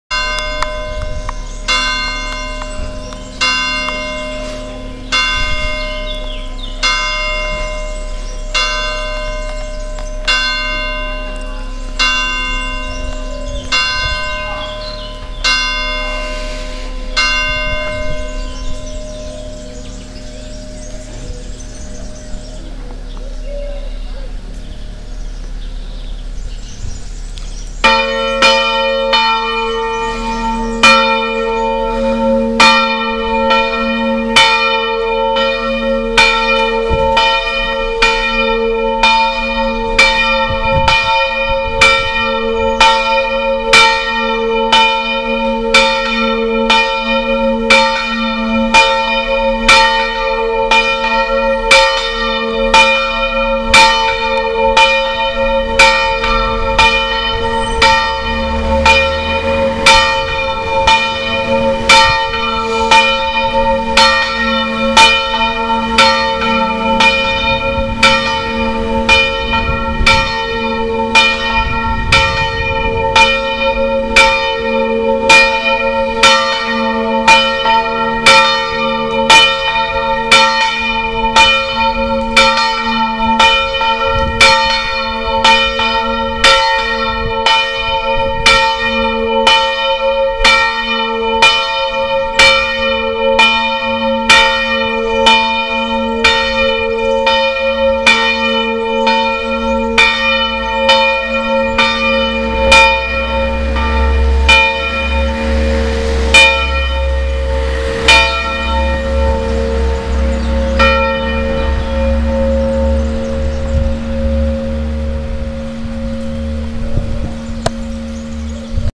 60 RONCITELLI FRAZ SENIGALLIA PARR.SAN GIOVANNI BATTISTA
battiore 12 e distesa 1° angelus festivo